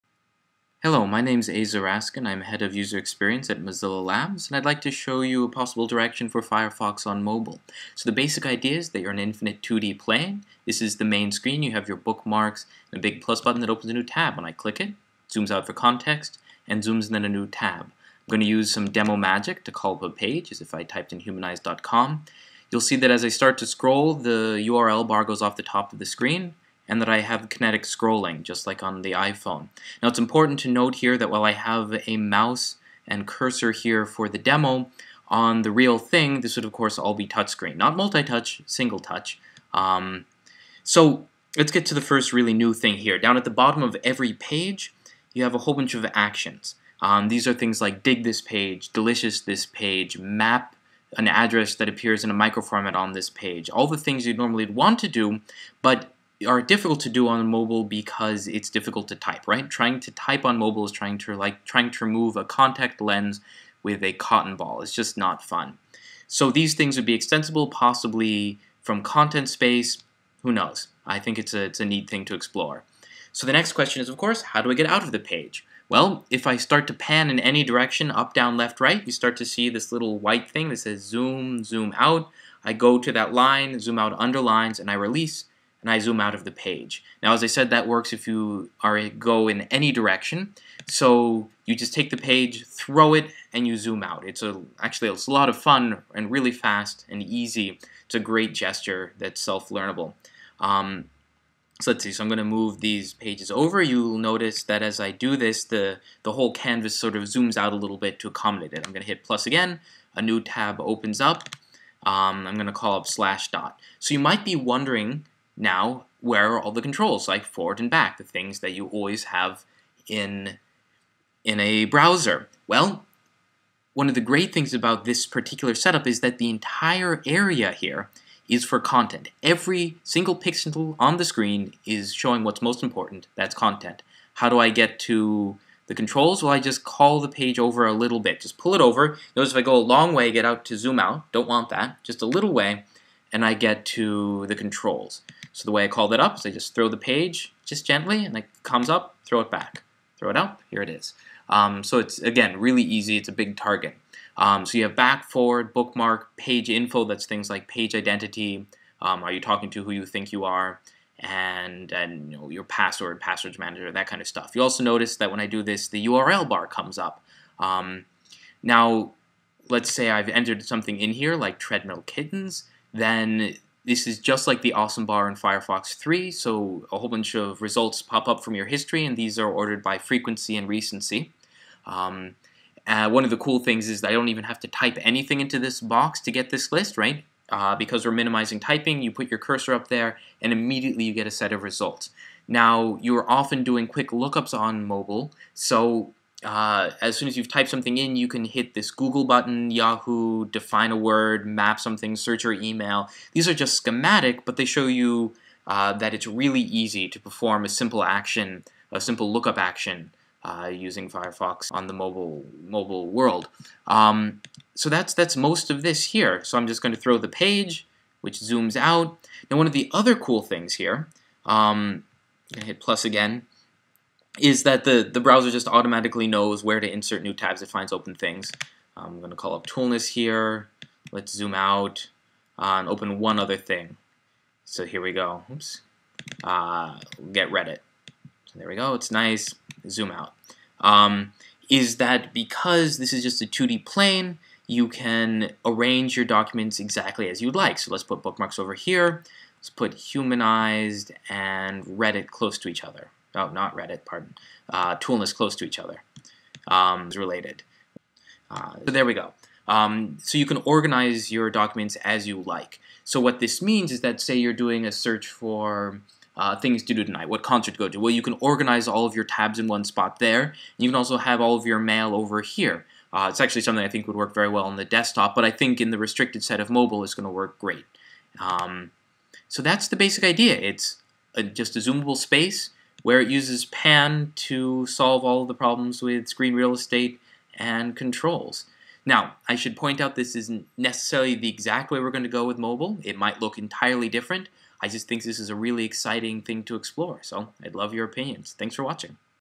Firefox Mobile Concept Demo
One possible direction for how mobile Firefox could look. Presented by Aza Raskin of Mozilla Labs.